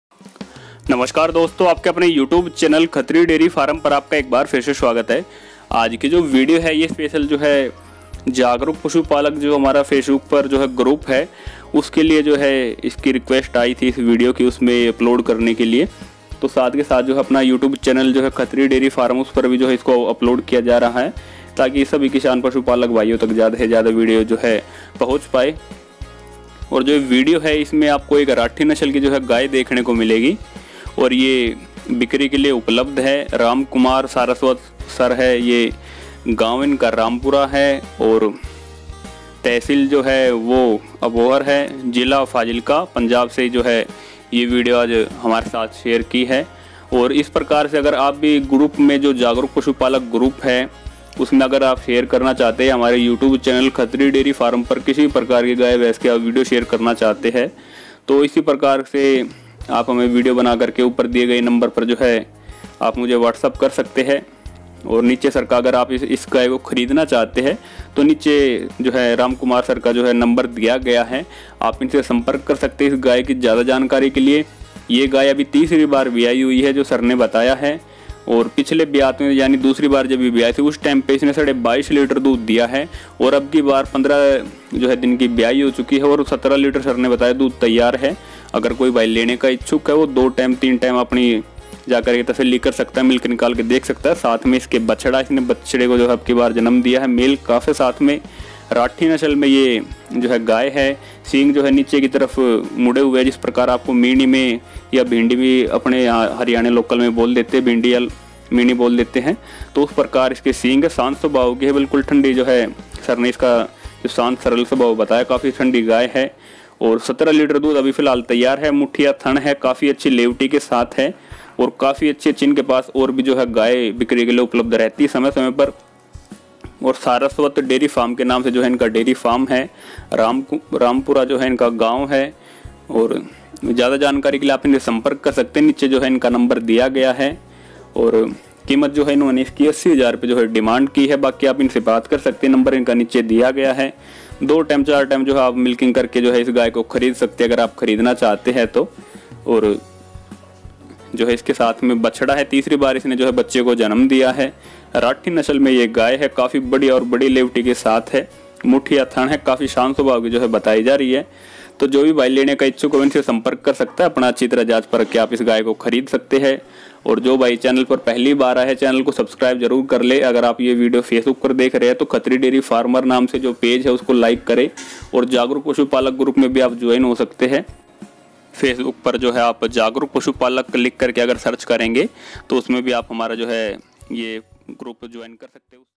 FOR SALE:~3RD TIMER RATHI COW 15 दिन की बियाई 17 लीटर दुध तैयार।सारस्वत डेयरी फार्म।रामपुरा,पंजाब
FOR SALE:~3RD TIMER RATHI COW 15 दिन की बियाई 17 लीटर दुध तैयार।सारस्वत डेयरी फार्म

सारस्वत डेयरी फार्म गांव रामपुरा तहसील अबोहर जिला फाजिल्का पंजाब 👏